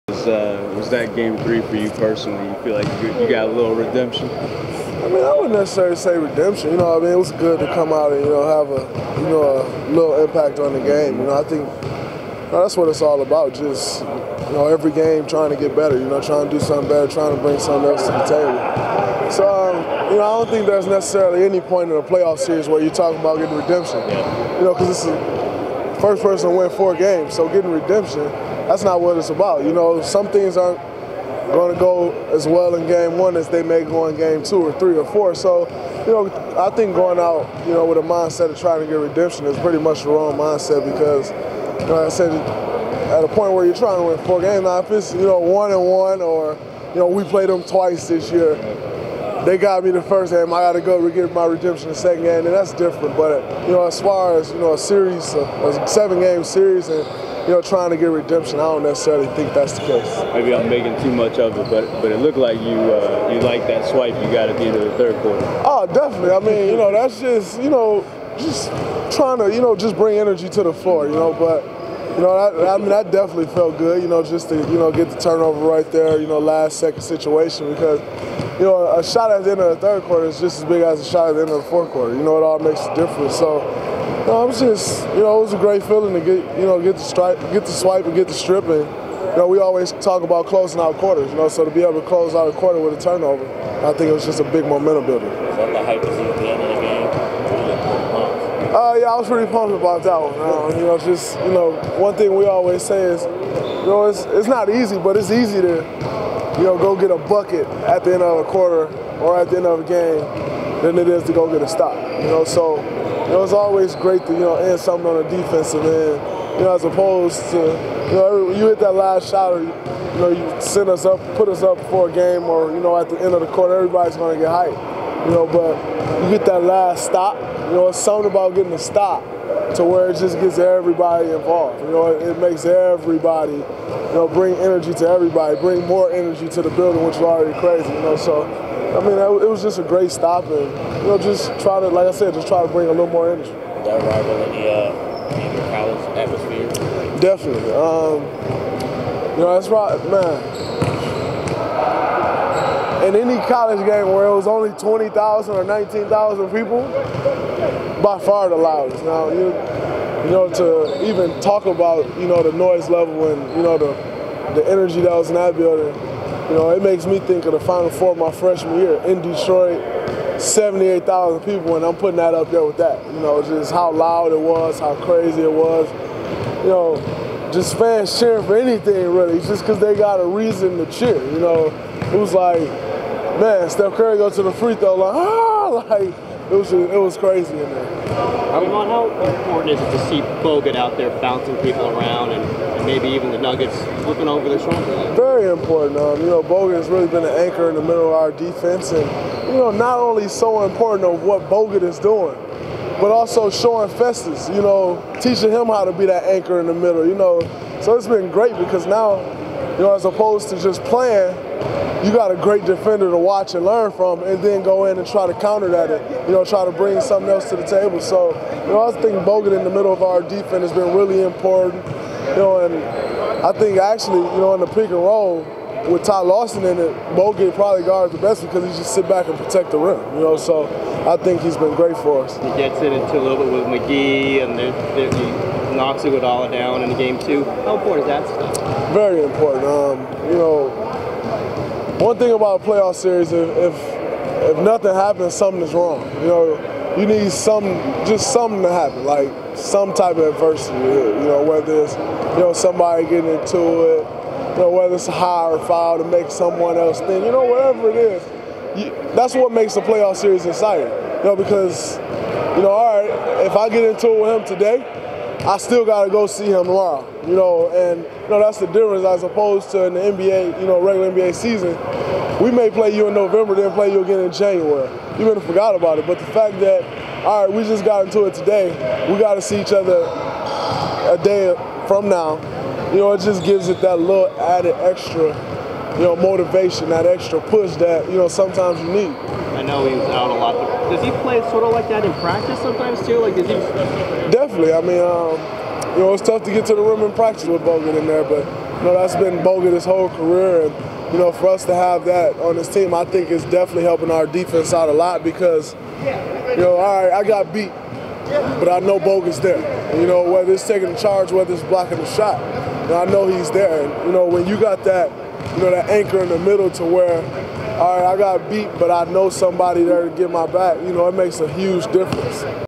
Practice: Draymond Green - 4/27/13
Warriors Forward Draymond Green talks with the media before today's practice.